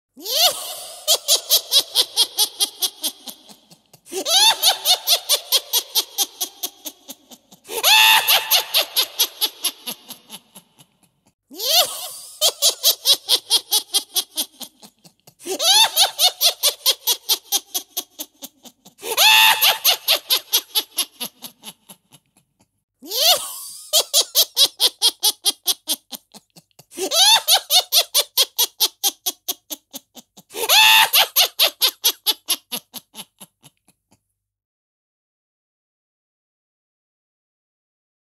Witch Laughter Sounds 🧹 The Sound of a Witch Laughing
The spooky sound of a witch laughing.

This the spooky and classic sound of a witch laughing. It's the perfect addition to any fun but terror filled halloween night, setting the mood for a halloween themed party, or scaring a few trick-or-treaters.